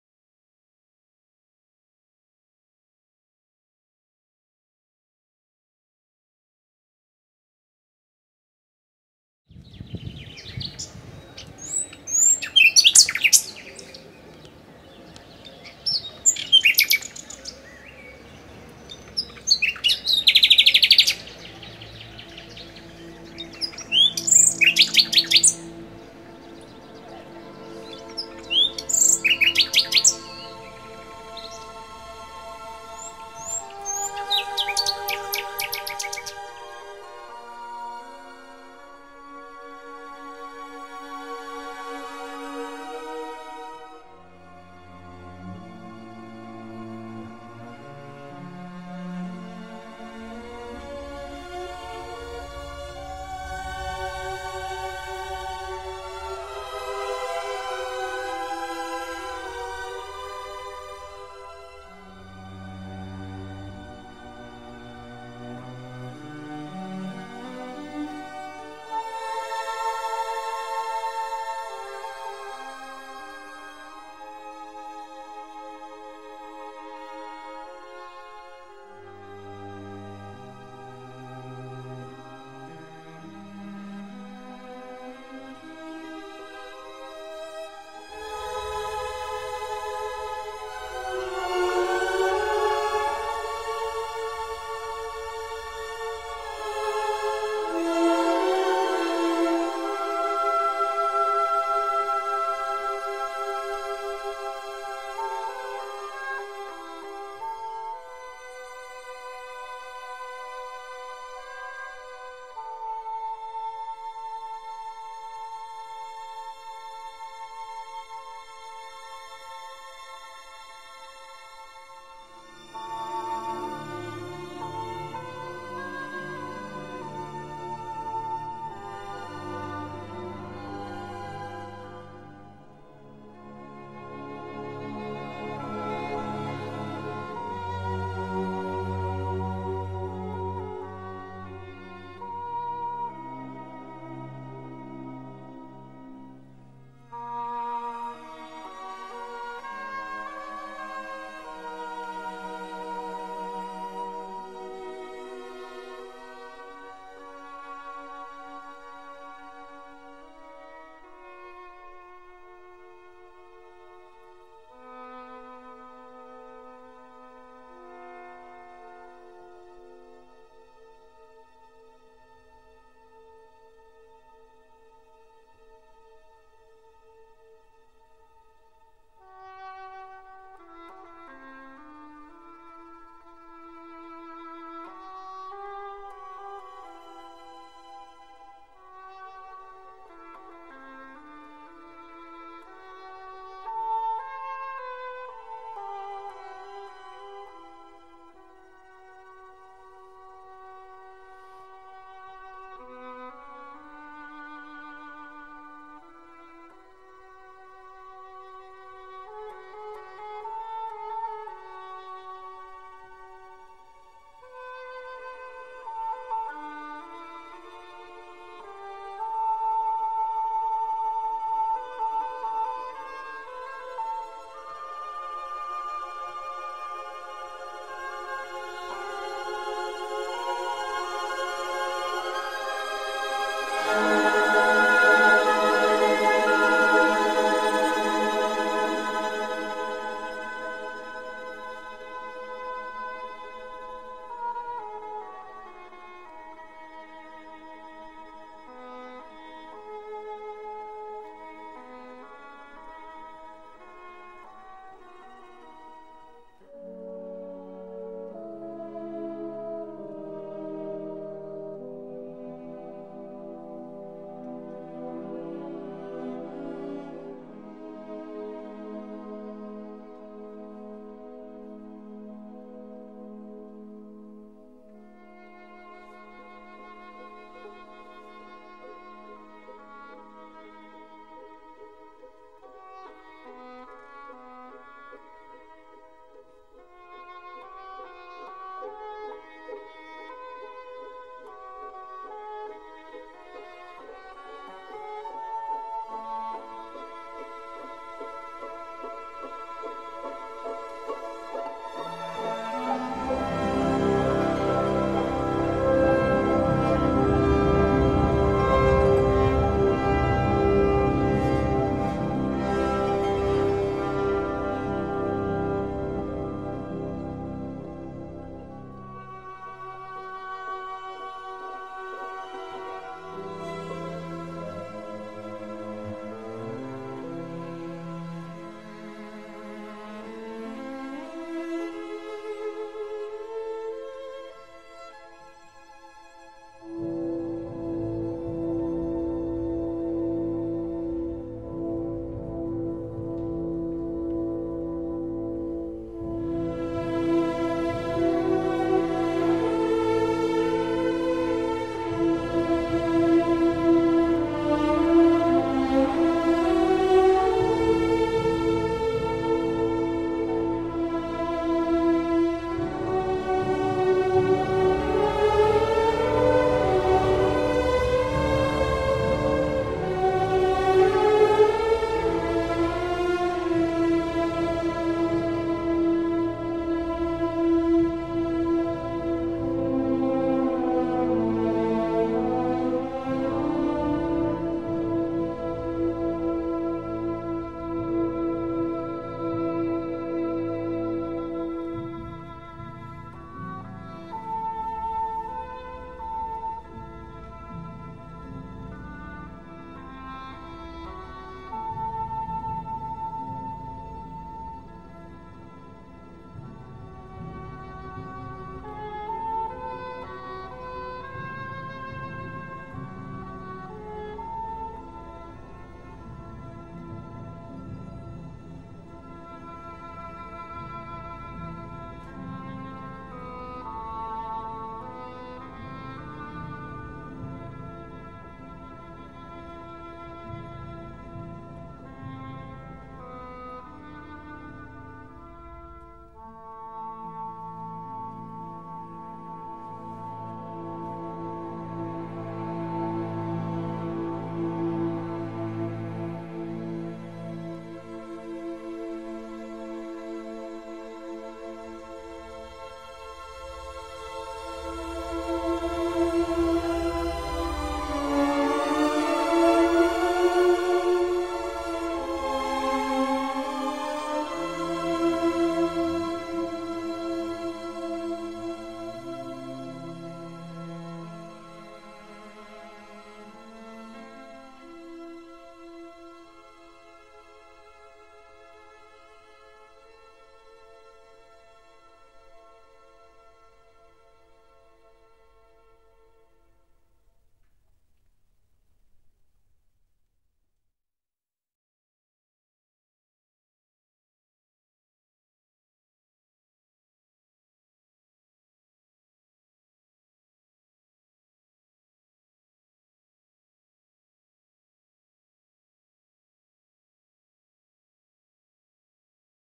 The Thracian Mysteries
A video material regarding work on Universal GameDev Challenge,showing locations,and Thracian artifacts, relating a future game project;The Angkor Wat scenes are authentic and original.